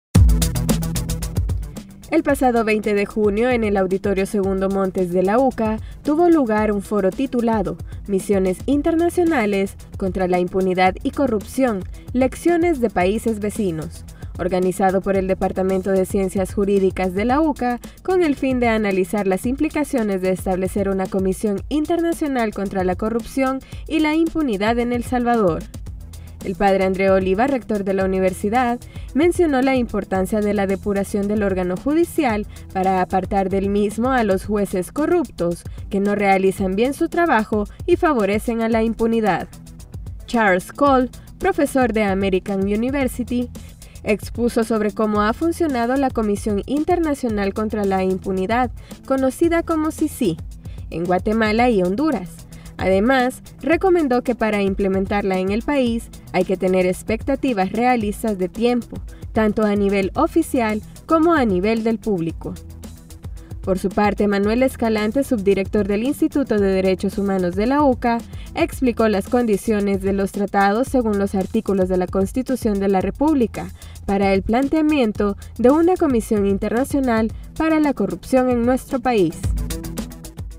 Nota: ¿Es posibles una CICIES en nuestro país?
El pasado 20 de junio en el auditorio Segundo Montes, tuvo lugar el foro titulado: “Misiones internacionales contra la impunidad y corrupción, lecciones de países vecinos”, organizado por el Departamento de Ciencias Jurídicas de la UCA, con el fin de analizar las implicaciones de establecer una comisión internacional contra la corrupción y la impunidad en El Salvador